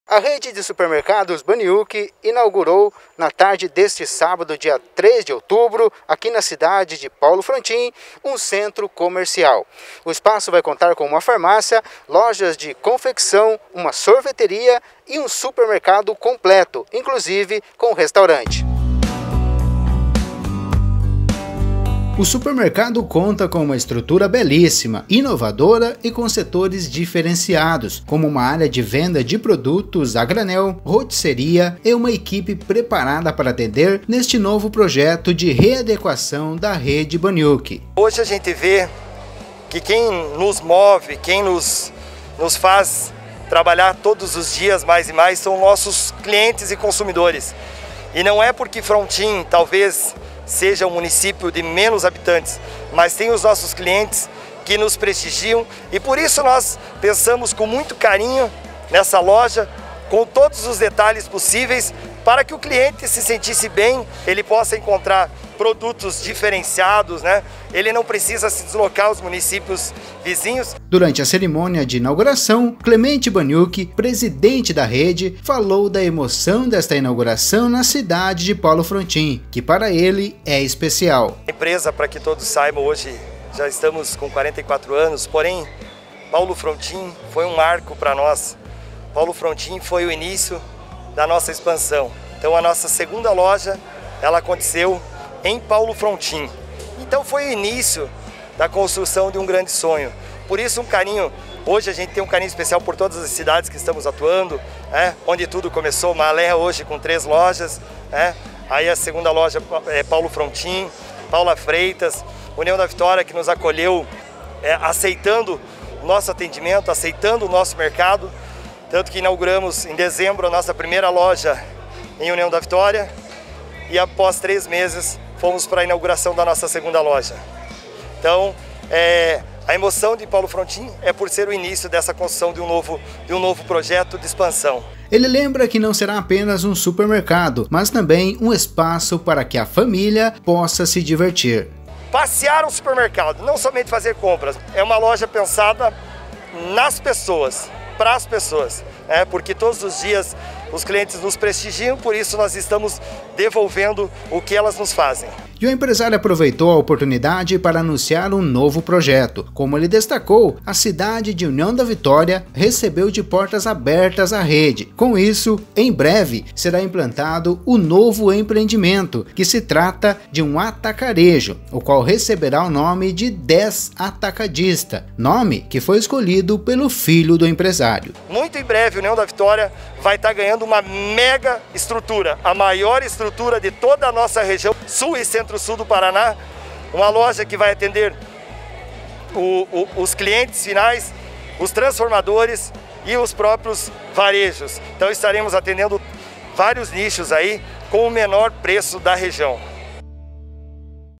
Rede de Supermercados Bahniuk inaugura Centro Comercial na cidade de Paulo Frontin
Durante a inauguração, empresário Clemente Bahniuk fala sobre a readequação da Rede e anuncia novo projeto para União da Vitória